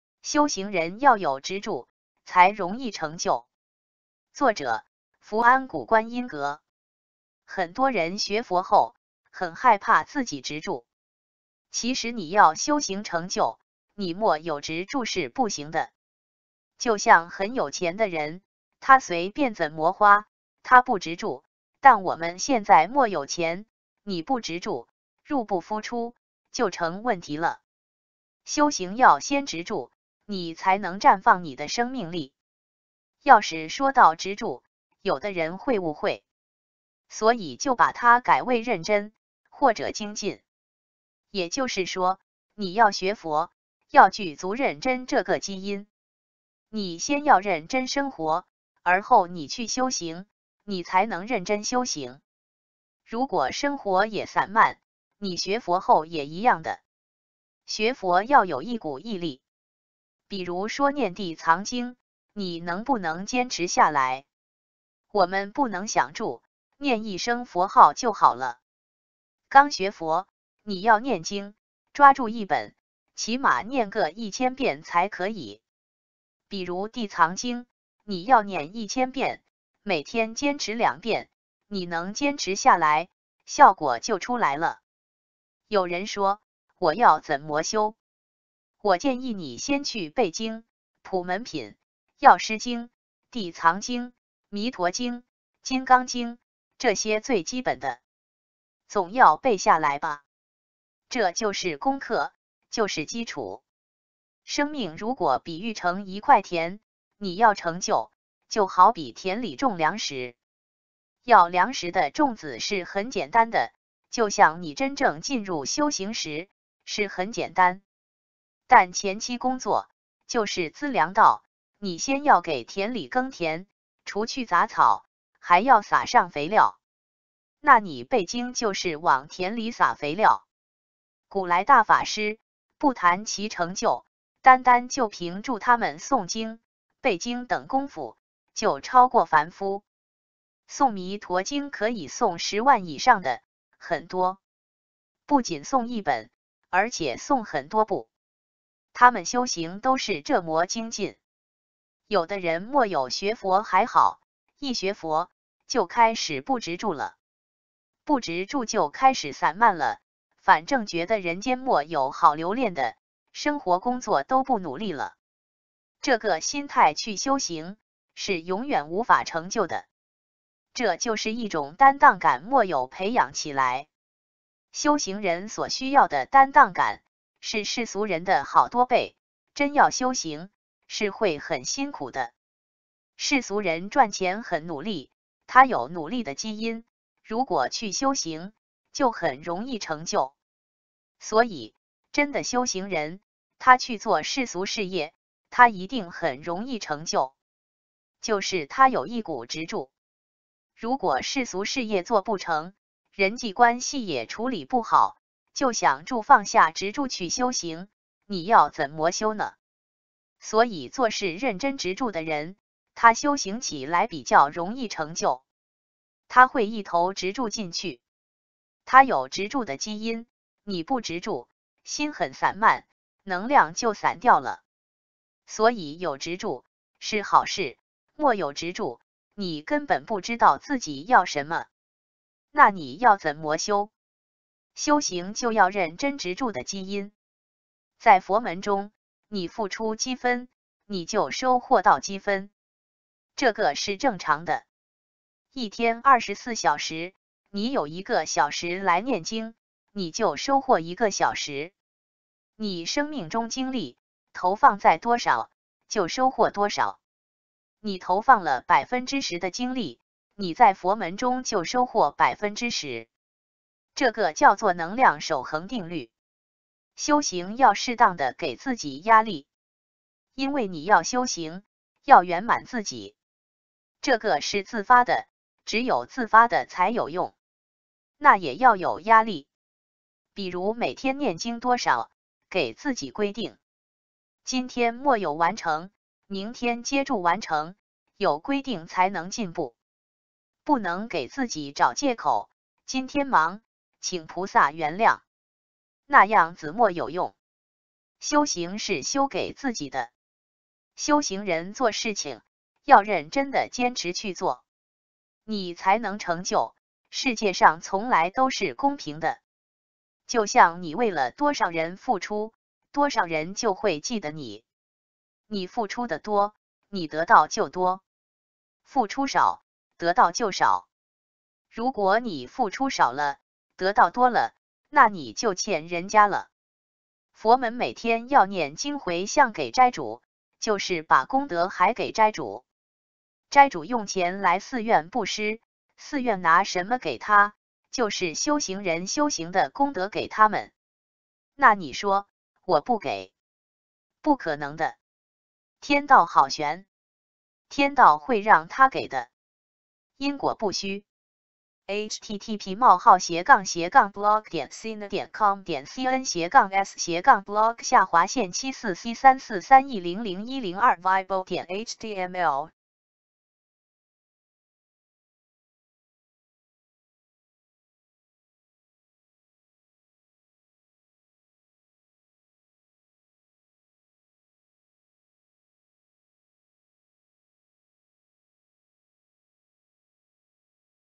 修行人要有執著，才容易成就
作者：福安古觀音閣

   很多人學佛後，很害怕自己執著。其實你要修行成就，你沒有執著是不行的。就像很有錢的人，他隨便怎麼花，他不執著，但我們現在沒有錢，你不執著，入不敷出，就成問題了。

   修行要先執著，你才能綻放你的生命力。要是說到執著，有的人會誤會。所以就把他改為認真，或者精進。也就是說，你要學佛，要具足認真這個基因。你先要認真生活，而後你去修行，你才能認真修行。如果生活也散漫，你學佛後也一樣的。

   學佛要有一股毅力。比如說念地藏經，你能不能堅持下來。我們不能想著，念一聲佛號就好了。剛學佛，你要念經，抓住一本，起碼念個一千遍才可以。比如地藏經，你要念一千遍，每天堅持兩遍，你能堅持下來，效果就出來了。

    有人說，我要怎麼修。我建議你先去背經，普門品，藥師經，地藏經，彌陀經，金剛經，這些最基本的。總要背下來吧。這就是功課，就是基礎。生命如果比喻成一塊田，你要成就，就好比田裡種糧食。要糧食的種子是很簡單的，就像你真正進入修行時，是很簡單。但前期工作，就是資糧道，你先要給田裡耕田，除去雜草，還要撒上肥料。那你背經就是往田裡撒肥料。

   古來大法師，不談其成就，單單就憑著他們誦經，背經等功夫，就超過凡夫。誦彌陀經可以誦十萬以上的，很多。不僅誦一本，而且誦很多部。他們修行都是這麼精進。

   有的人沒有學佛還好，一學佛，就開始不執著了。不執著就開始散漫了，反正覺得人間沒有好留戀的，生活工作都不努力了。這個心態去修行，是永遠無法成就的。這就是一種擔當感沒有培養起來。修行人所需要的擔當感，是世俗人的好多倍，真要修行，是會很辛苦的。

   世俗人賺錢很努力，他有努力的基因，如果去修行，就很容易成就。所以，真的修行人，他去做世俗事業，他一定很容易成就。就是他有一股執著。如果世俗事業做不成，人際關係也處理不好，就想著放下執著去修行，你要怎麼修呢？

    所以做事認真執著的人，他修行起來比較容易成就。他會一頭執著進去。他有執著的基因，你不執著，心很散漫，能量就散掉了。所以有執著，是好事，沒有執著，你根本不知道自己要什麼？那你要怎麼修？

    修行就要認真執著的基因。在佛門中，你付出幾分，你就收獲到幾分。這個是正常的。一天二十四小時，你有一個小時來念經，你就收獲一個小時。你生命中精力，投放在多少，就收獲多少。你投放了百分之十的精力，你在佛門中就收獲百分之十。這個叫做能量守恆定律。

   修行要適當的給自己壓力。因為你要修行，要圓滿自己。這個是自發的，只有自發的才有用。那也要有壓力。比如每天念經多少，給自己規定。今天沒有完成，明天接著完成，有規定才能進步。不能給自己找藉口，今天忙，請菩薩原諒。那樣子沒有用。修行是修給自己的。

   修行人做事情，要認真的堅持去做。你才能成就，世界上從來都是公平的。就像你為了多少人付出，多少人就會記得你。你付出的多，你得到就多。付出少，得到就少。如果你付出少了，得到多了，那你就欠人家了。

  佛門每天要念經回向給齋主，就是把功德還給齋主。齋主用錢來寺院佈施，寺院拿什麼給他，就是修行人修行的功德給他們。那你說，我不給。不可能的。天道好還。天道會讓他給的。因果不虛。